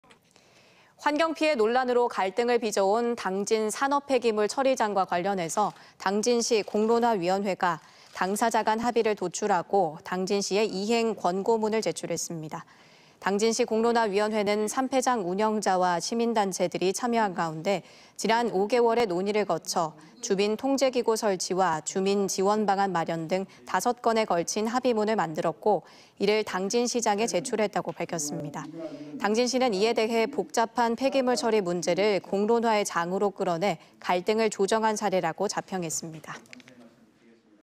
당진공론화위, 5개월 논의 끝에 산업폐기물 처리장 합의｜ TJB 대전·충남·세종뉴스
환경피해 논란으로
갈등을 빚어온
당진 산업폐기물
처리장과 관련해 당진시 공론화위원회가
당자자간 합의를 도출하고
당진시에 이행 권고문을 제출했습니다.

당진시 공론화위원회는
산폐장 운영자와 시민단체들이 참여한 가운데
지난 5개월의 논의를 거쳐
주민 통제기구 설치와 주민 지원방안 마련 등
5건에 걸친 합의문을 만들었고
이를 당진시장에 제출했다고 밝혔습니다.

당진시는 이에 대해
복잡한 폐기물 처리 문제를
공론화의 장으로 끌어내
갈등을 조정한 사례라고 자평했습니다.